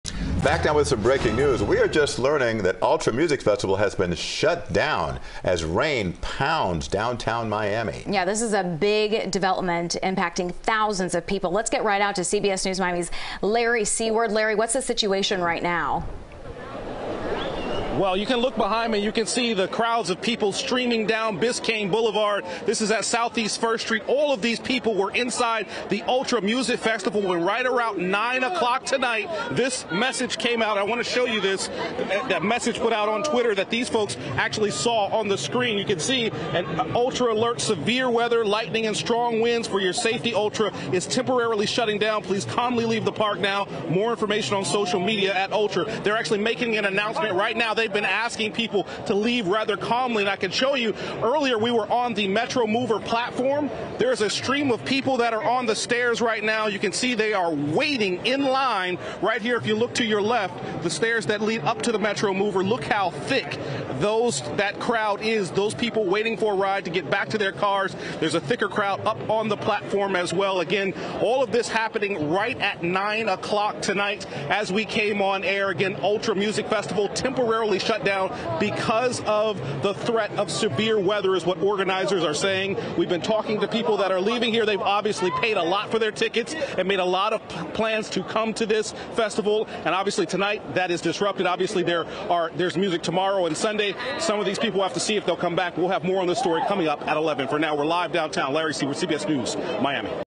Ultra Music Festival shuts down for the night due to weather
Ultra Music Festival shuts down for the night due to weather to the disappointment of thousands.